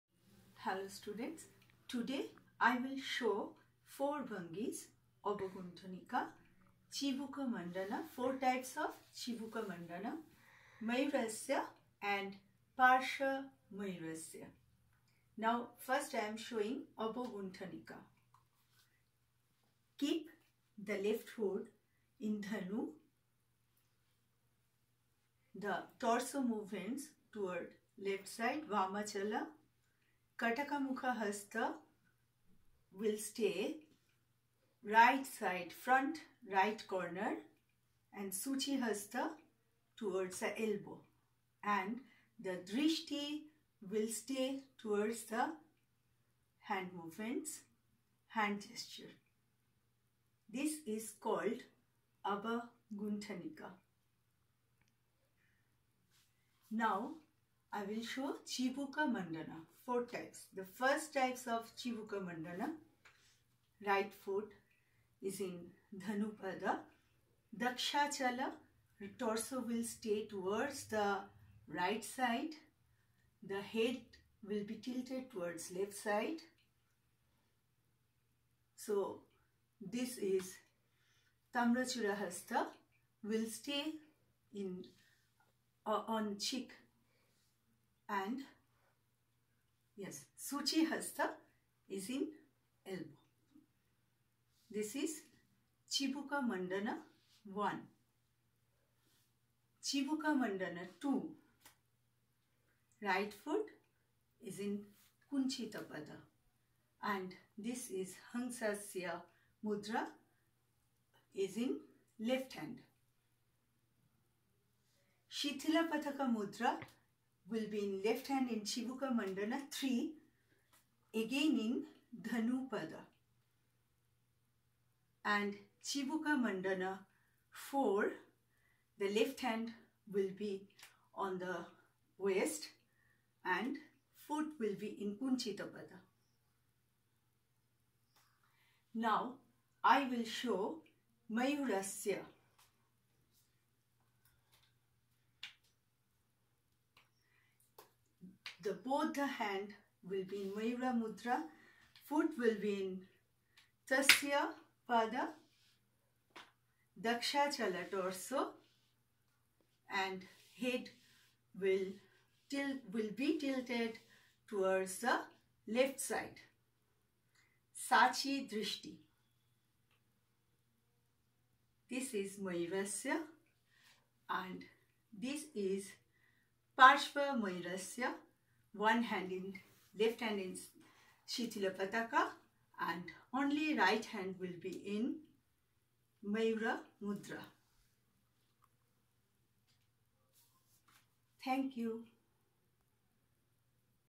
Bhangi- 13-16, session 28, Junior Class,St Xavier's Institution
basic postures of Odissi Dance